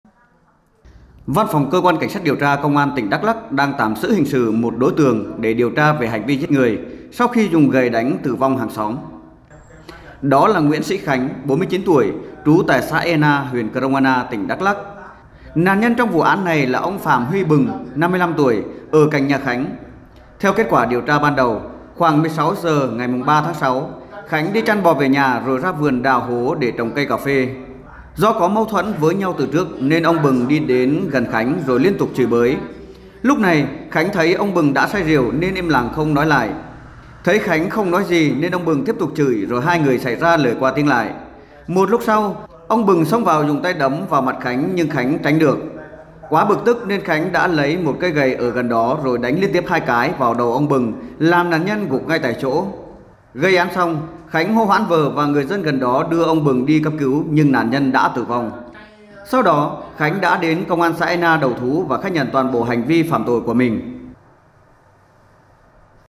Nóng: Bắt đối tượng đánh hàng xóm tử vong do liên tục bị chửi bới
Văn phòng cơ quan Cảnh sát điều tra Công an tỉnh Đắk Lắk đang tạm giữ hình sự đối tượng Nguyễn Sỹ Khánh (49 tuổi) trú tại xã Ea Na, huyện Krông Ana, tỉnh Đắk Lắk để điều tra về hành vi giết người.